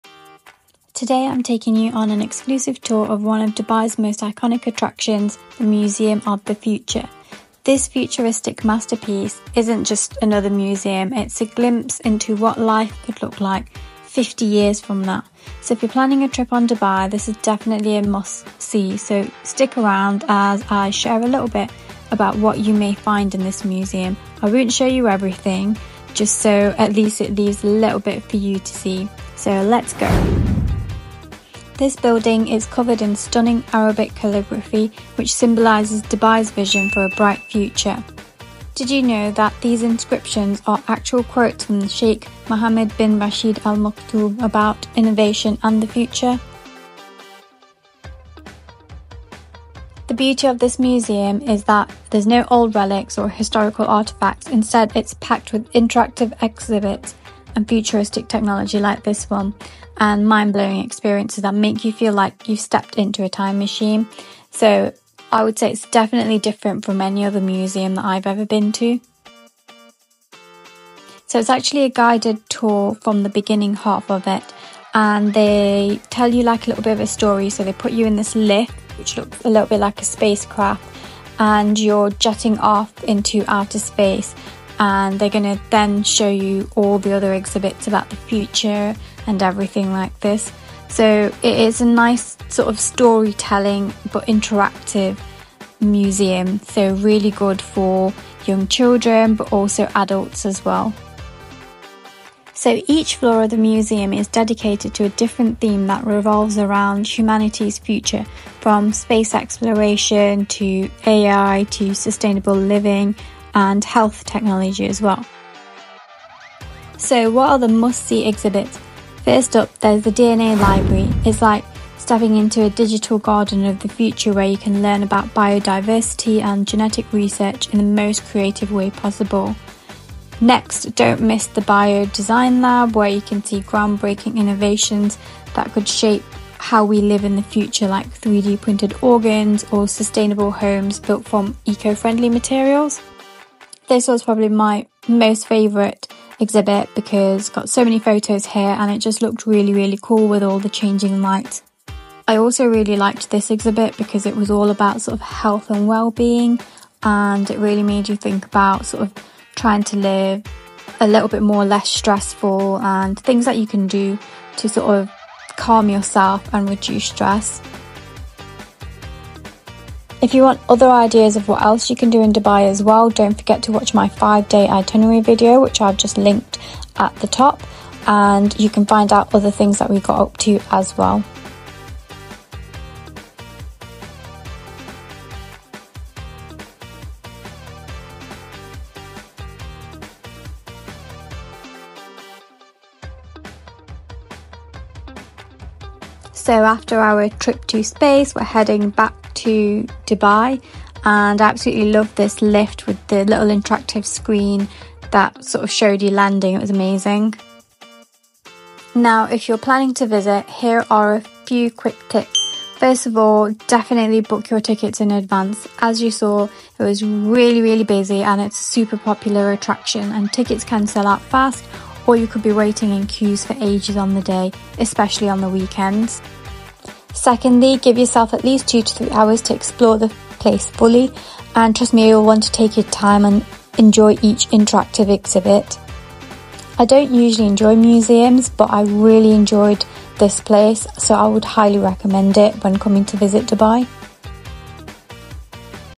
I Explore Museum of the Future Dubai| Most Futuristic Attraction 2024
Welcome to my quick guide to the Museum of the Future in Dubai – one of the city’s most exciting and futuristic attractions! In this video, I take you on a tour through the museum’s interactive exhibits, mind-blowing technology, and cutting-edge design. If you're visiting Dubai in 2024 or 2025, this is a must-see destination on your itinerary!

Whether you’re a tech enthusiast, a traveler, or just curious about the future, this video covers everything you need to know about the Museum of the Future – from the iconic architecture to the incredible exhibits on AI, space exploration, and sustainability.

📍 In this video, you’ll discover:

The amazing architecture of the Museum of the Future and its symbolic Arabic calligraphy
What to expect from the futuristic exhibits that explore life 50 years from now
The top exhibits you can’t miss, like the DNA Library and Bio-Design Lab
Essential tips for visiting the museum, including ticket booking and best times to visit
Insider insights into how the museum showcases technology, innovation, and the future of humanity
🌟 Why Visit the Museum of the Future?
The Museum of the Future in Dubai is more than just a tourist spot – it’s an immersive experience that challenges you to think about the possibilities for the world of tomorrow. From space travel to bio-engineering, it’s a journey through some of the most innovative technologies that will shape our future. Whether you’re planning your first trip to Dubai or a return visit, make sure to add this to your itinerary!

📍 Plan Your Visit: Location: Sheikh Zayed Road, Dubai, UAE
Opening Hours: Daily from 10 AM to 6 PM